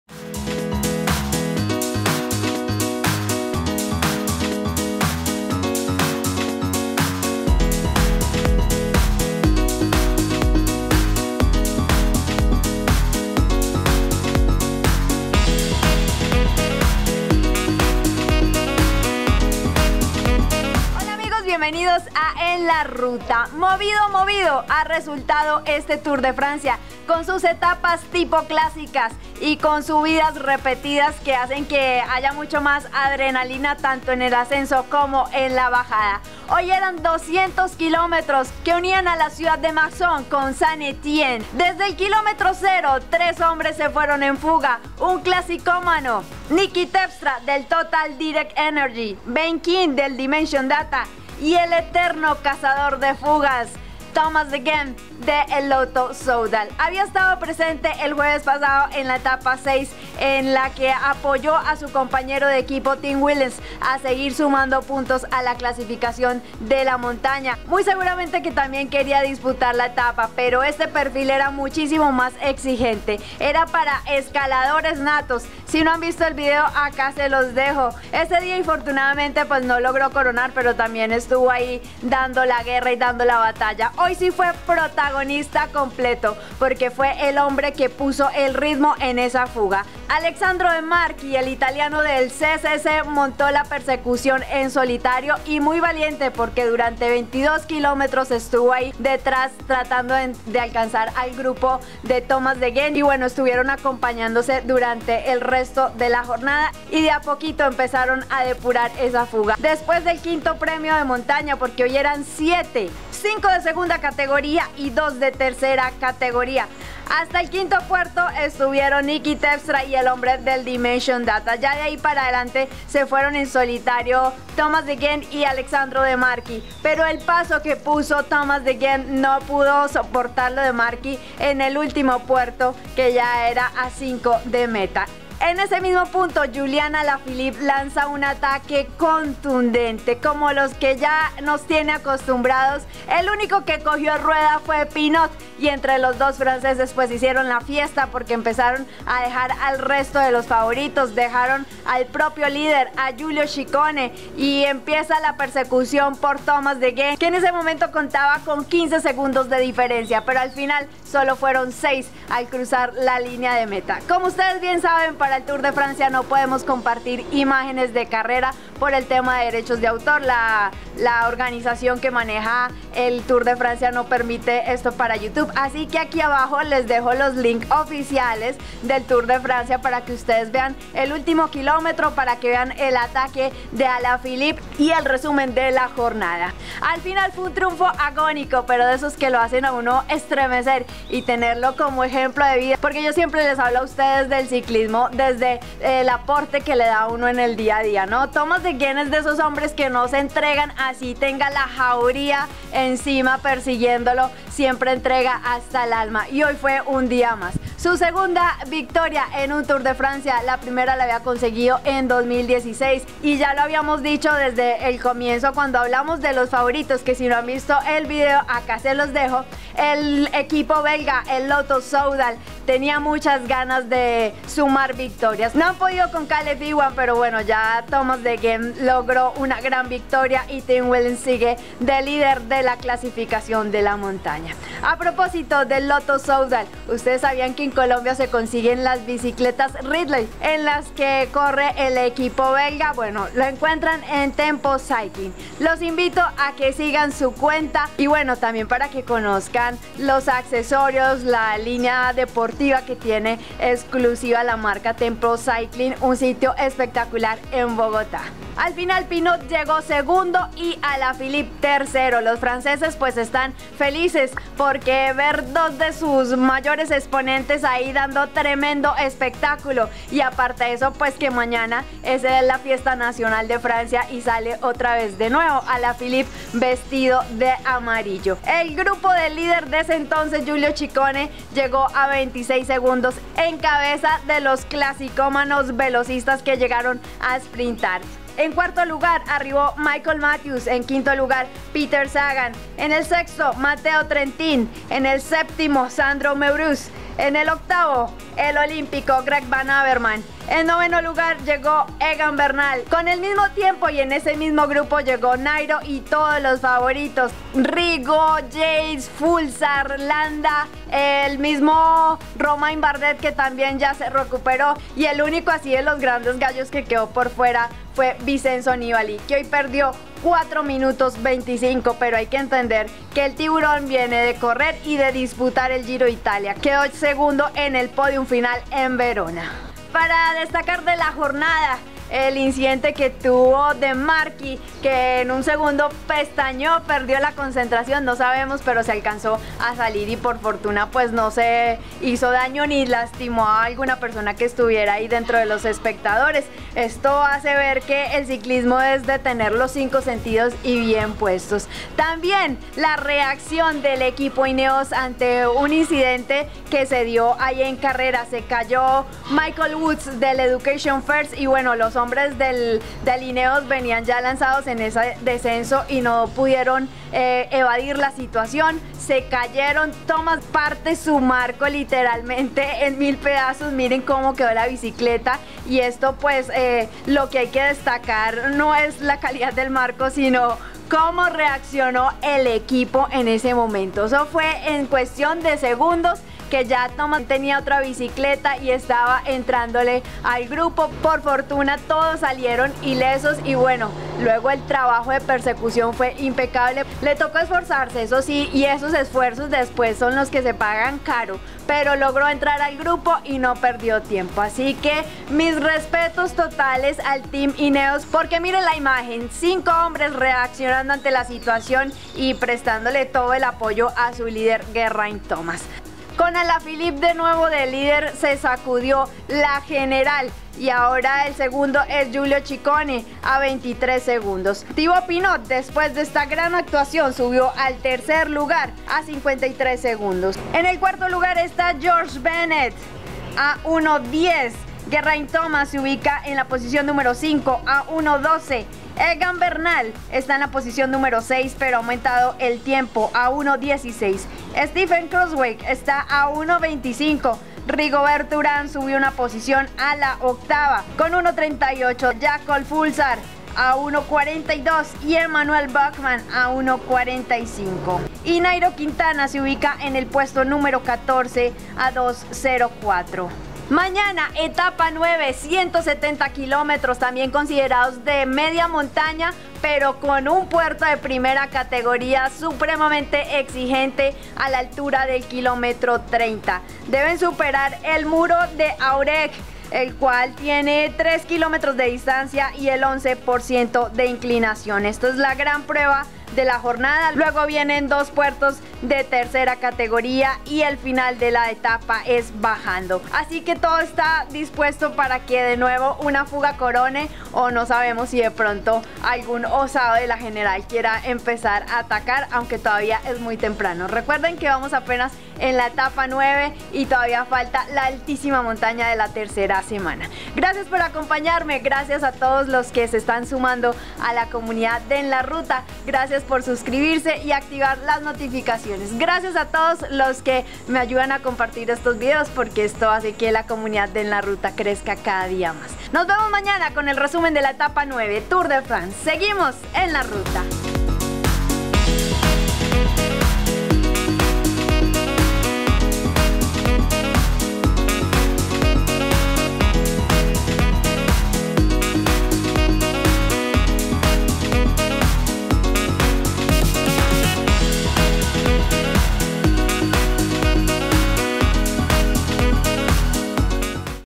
De Gendt corona después de 200 kms en fuga - Etapa 8 TDF 2019
Thomas de Gendt un especialista en cazar fugas hoy se hizo la más larga e importante de su carrera deportiva desde el kilómetro 0 poniendo ritmo y soportando la presión de tener encima a dos monstruos como Pinot y Alaphilippe que recuperó el Malliot Amarillo · Conozca todos los detalles de lo ocurrido en la etapa8 TourdeFrance · Gracias por Acompañarme

- VIDEO OFICIAL TOUR DE FRANCE -